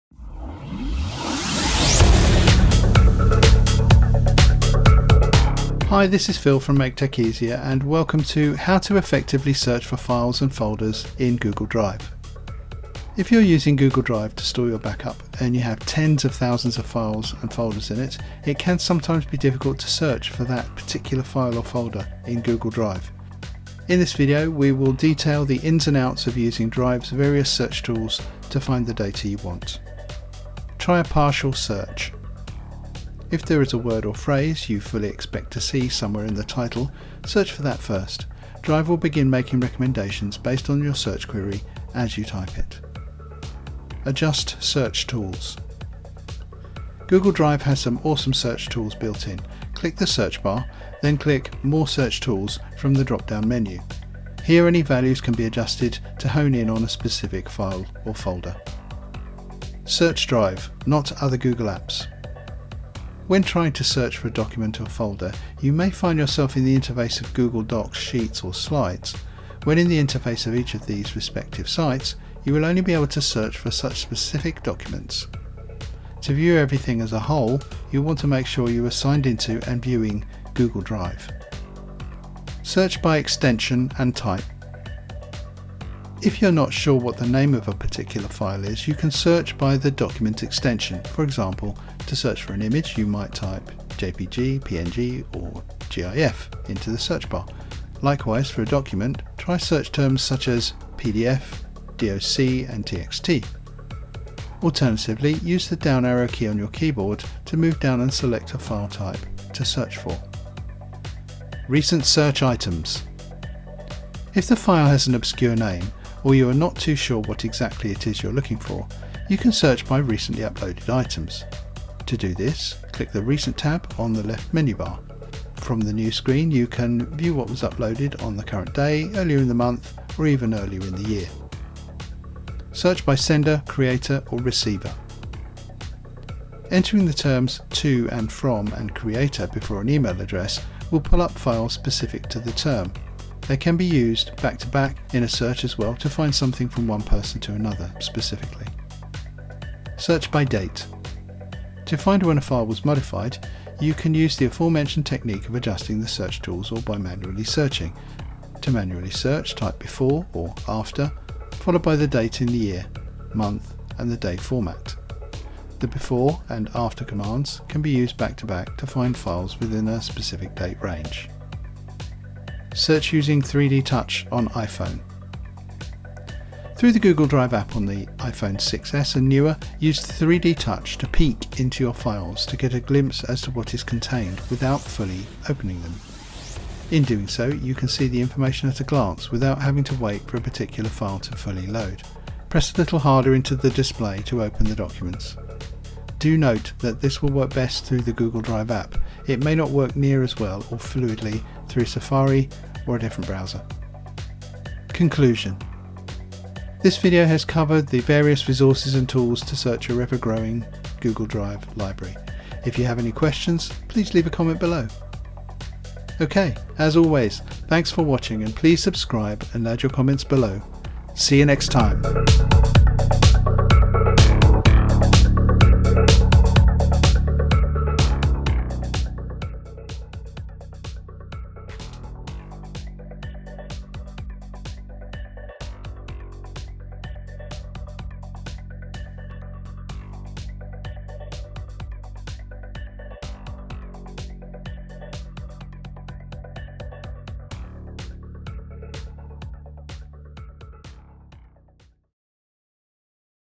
How to Effectively Search for Files and Folders in Google Drive
Doing a file search for folders and files on Google Drive can be a pain. Here are some tips to make it easier.
Full transcript on the caption/CC button.